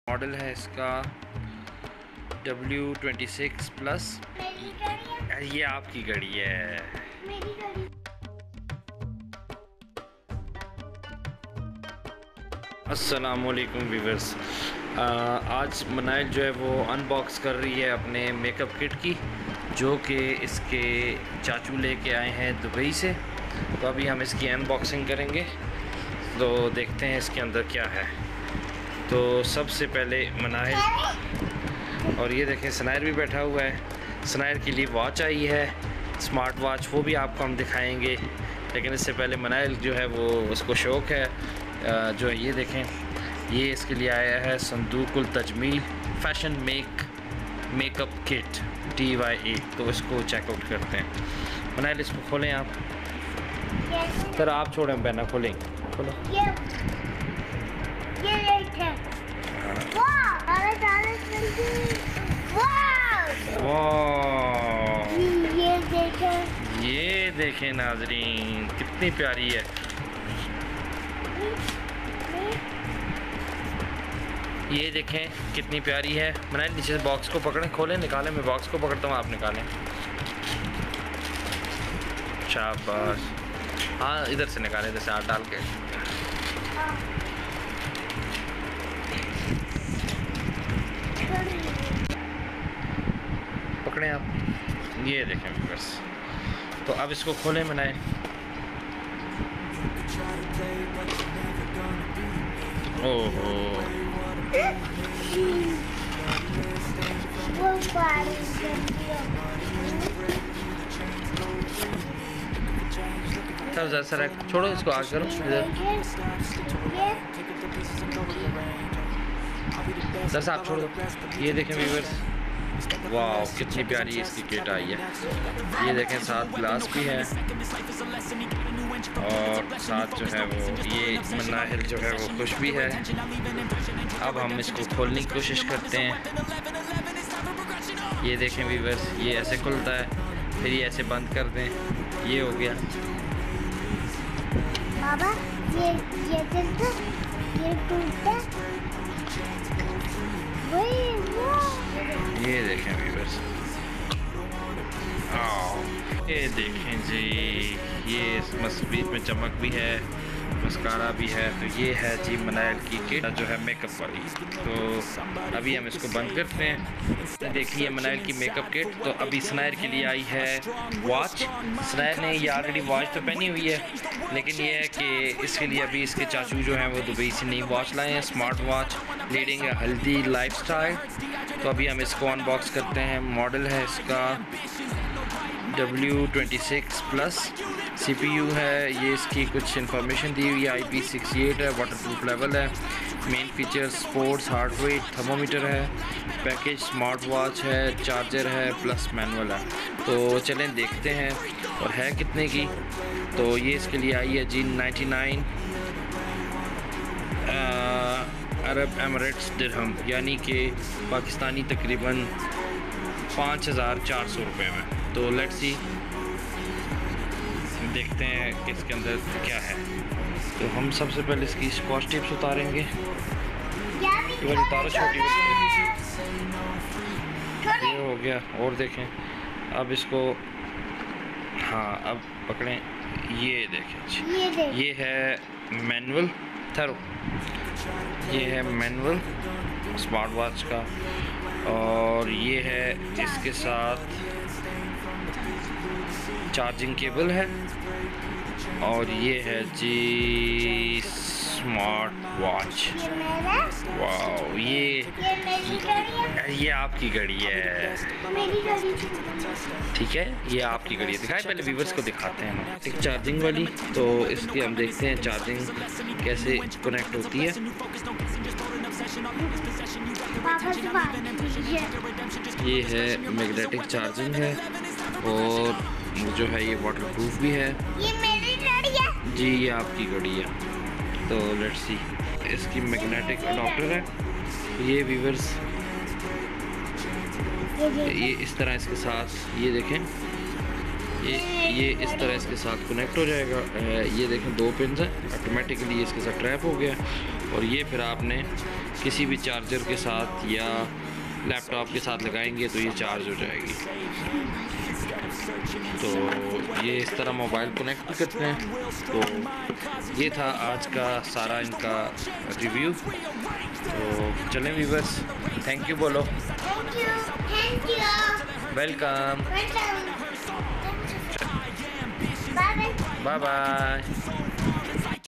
Unboxing Makeup Kit | Smart watch W26 Plus | Gifts | sanair manahil vlog
hello
In todays vlog u will se the gifts brought by Kids Chachu from Dubai